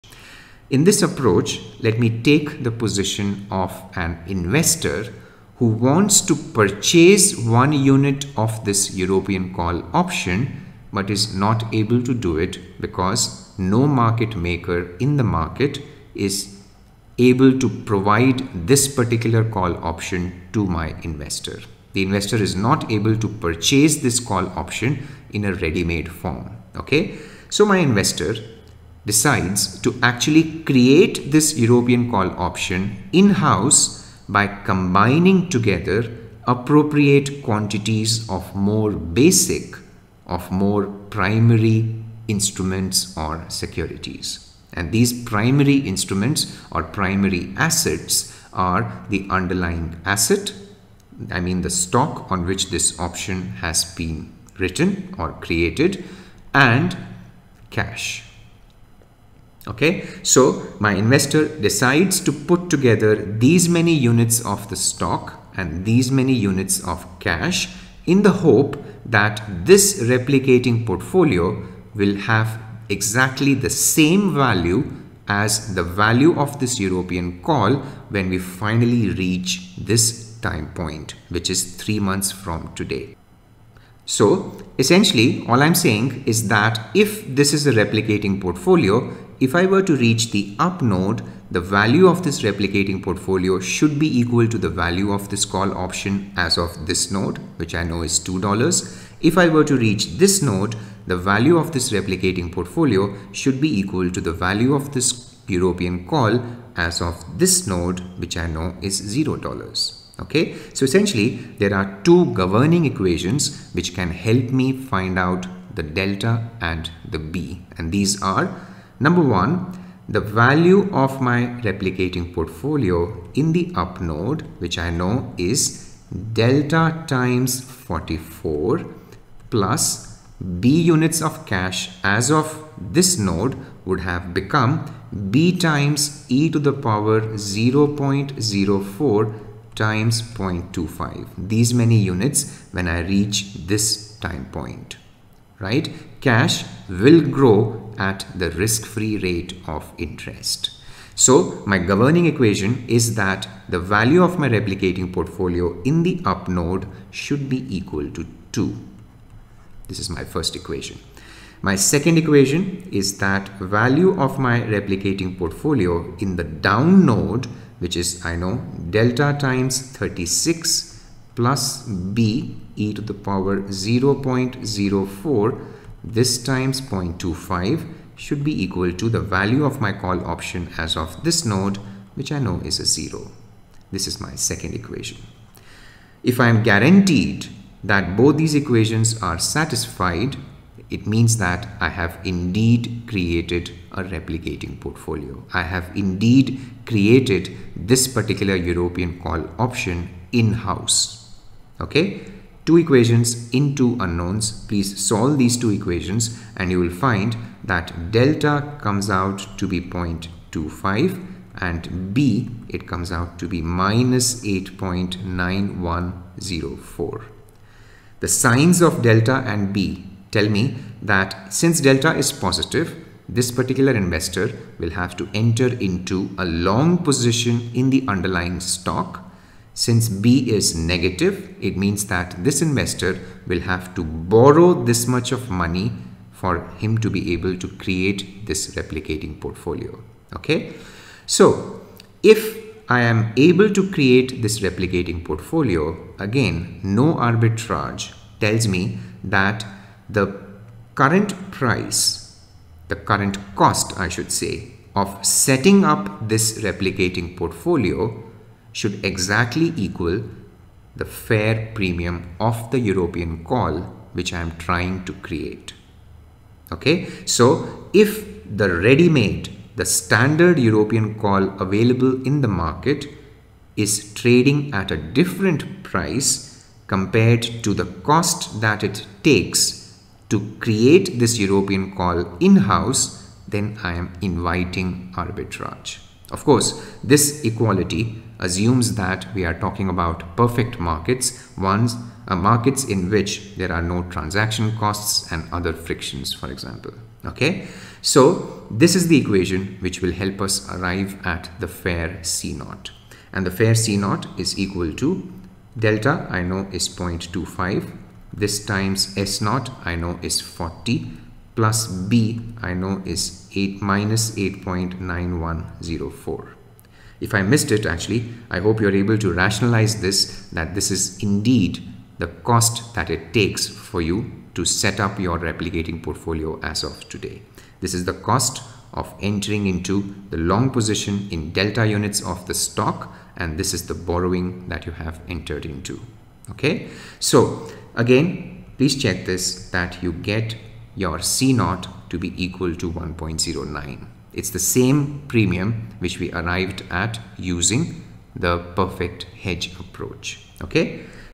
Binomial Option Pricing Model (Replicating Portfolio Approach) | FRM Part 1
In this video, we take a look at the Binomial option pricing model using a simple solved example. This example helps us confirm the equivalence of three approaches: Perfect Hedge approach, Replicating portfolio approach and Risk Neutral valuation approach.

This video is Part 2 of 3 videos on the Binomial Option Pricing Model. For more preparation resources related to the FRM Part 1 exam, please visit the course